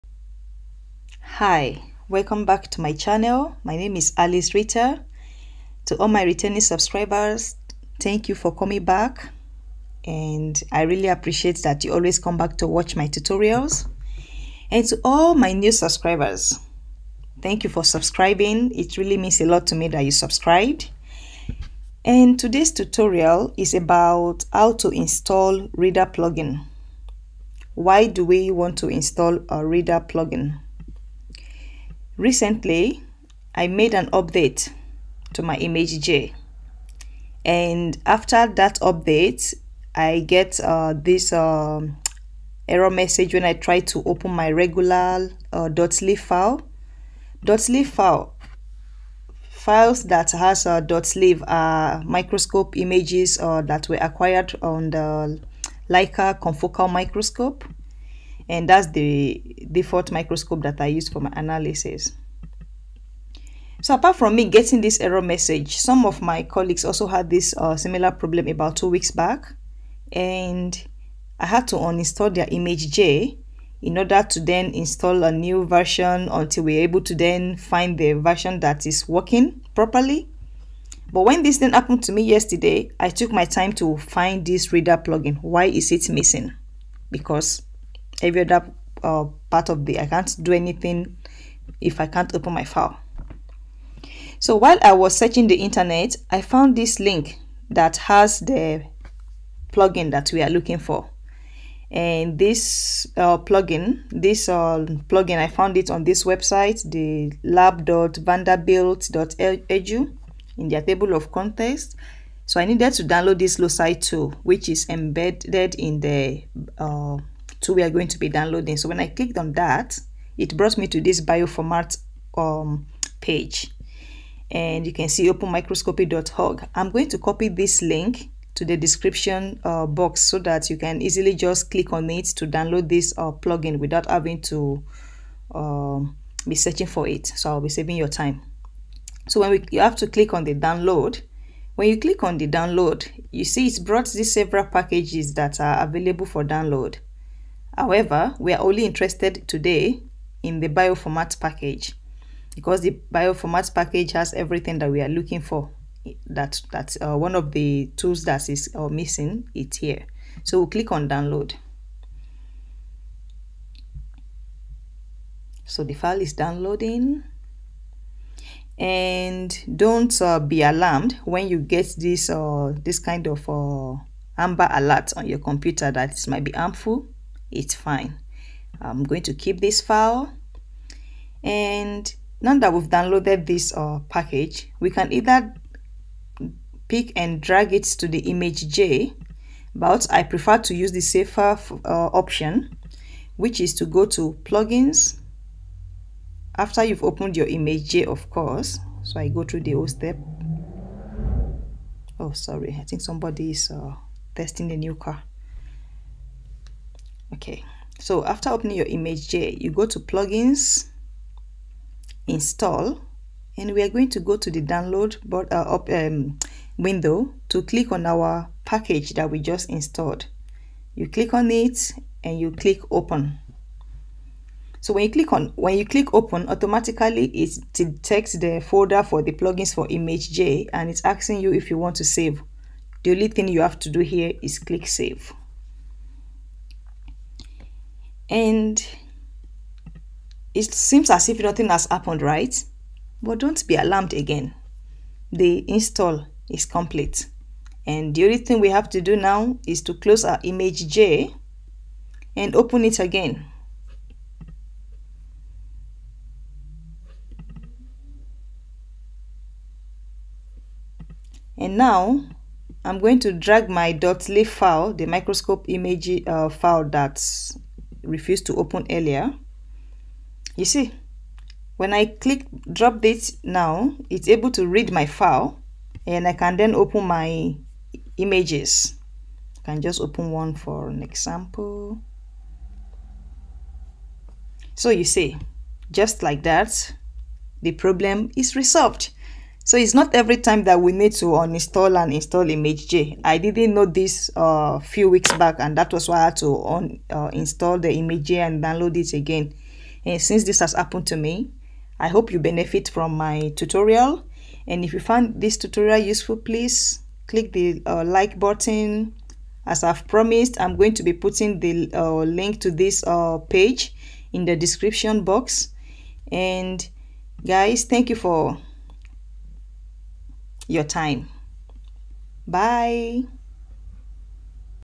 How to install ImageJ reader plugin| open Leica microscope file with imageJ| reader plugin not found
In this tutorial, you will learn how to install the reader plugin in ImageJ. Recently, I wanted to open .lif files in ImageJ and I got an error message stating "format not supported/reader plugin not found.  This has also happened to two of my colleagues and it seems it might be a common problem ImageJ users are currently experiencing. This tutorial shows you how to resolve the problem without having to uninstall the image.